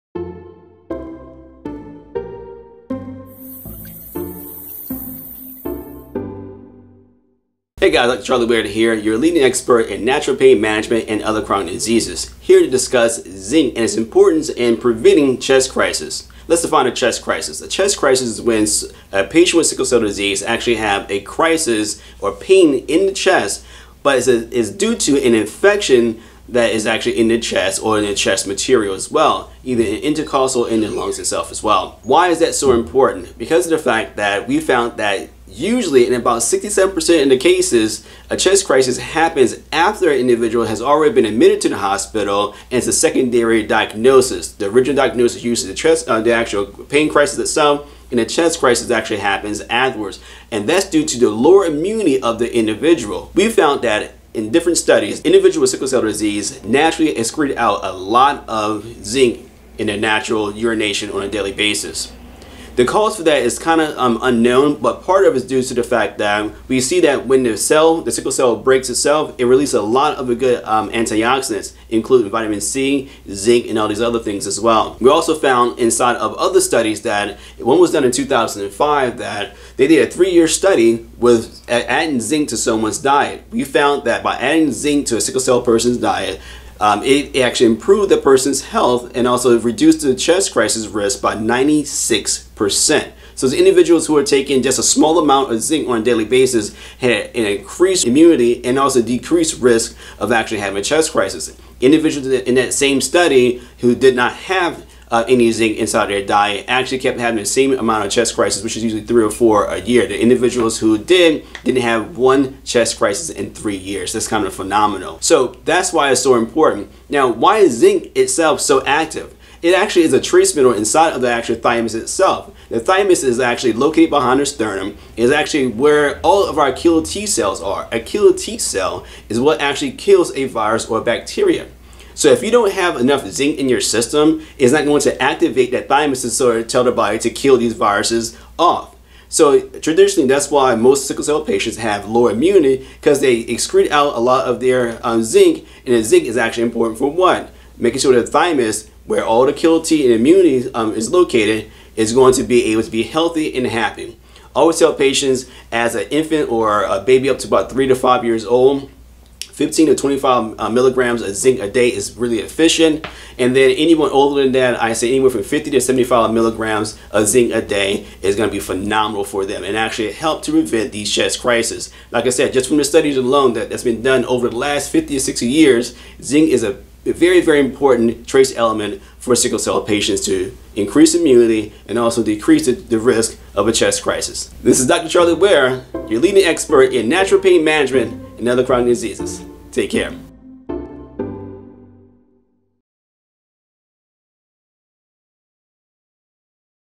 The Importance of Zinc in Preventing Chest Crisis for Sickle Cell Patients
In this video, Dr Charlie Ware discusses how important zinc plays in helping to prevent chest crisis in sickle cell patients.